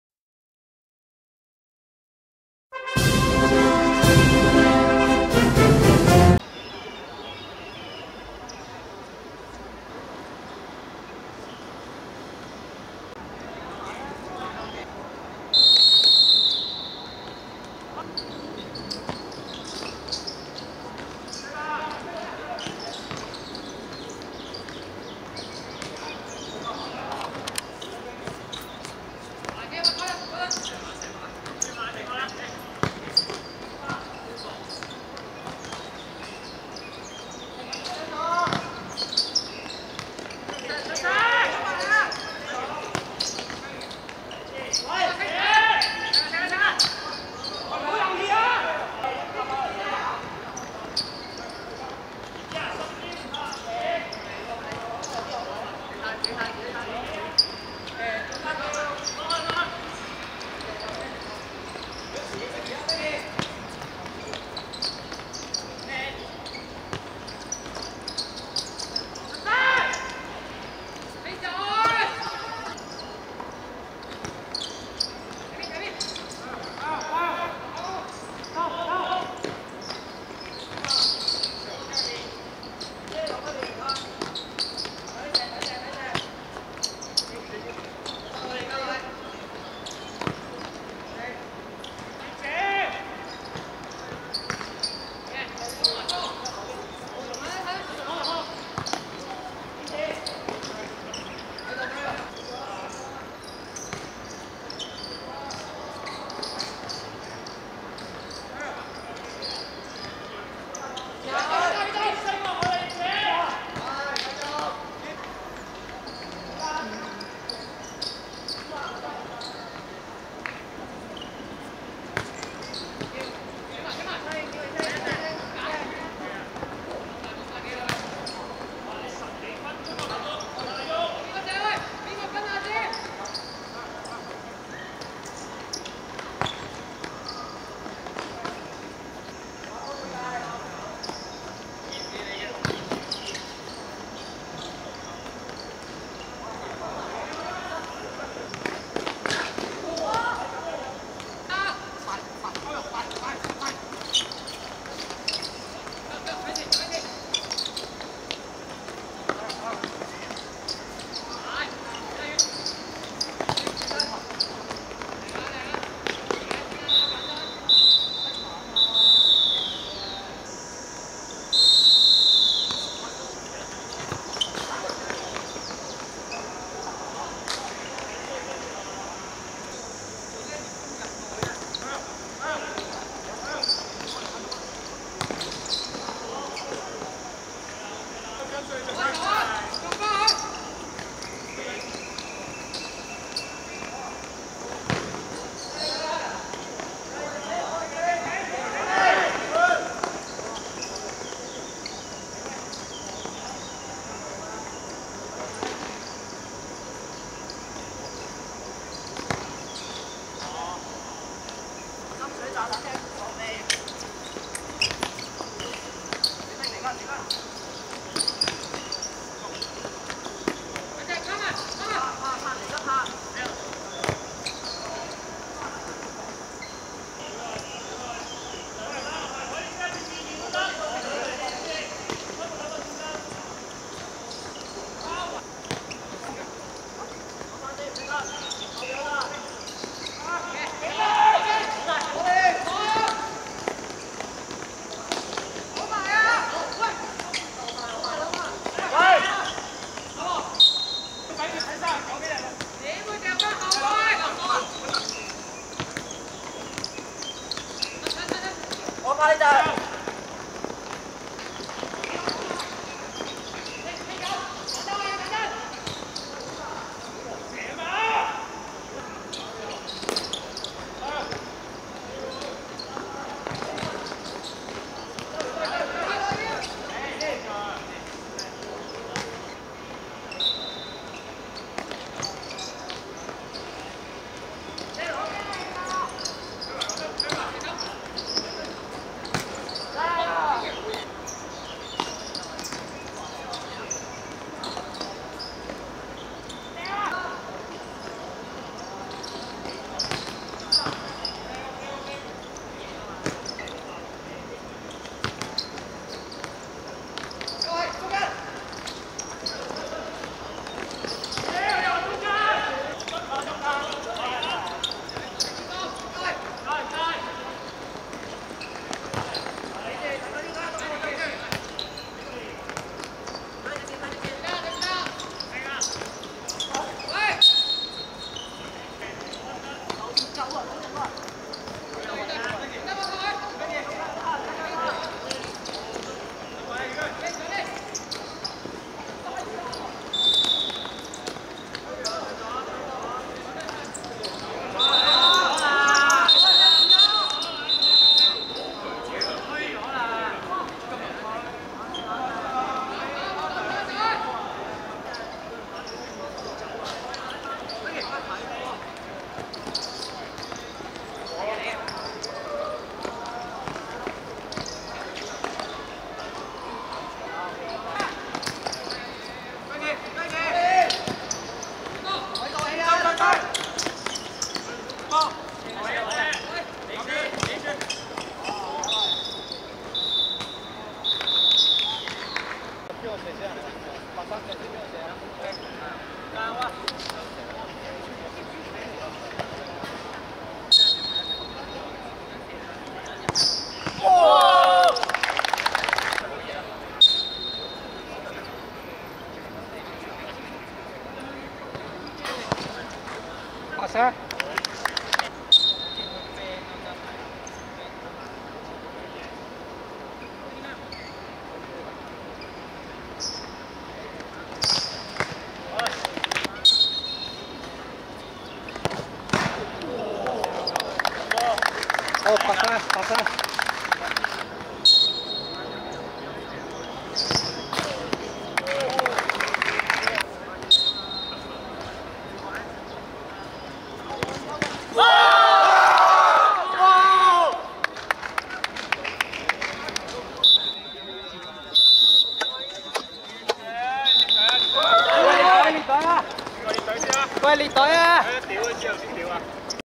英華vs安柱(2023.7.24.葵青區國慶盃足球賽~中學組十六強)精華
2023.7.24.青衣公園球塲 18:00
英華(黃)~(2) 0 : 0 (3)~安柱(白)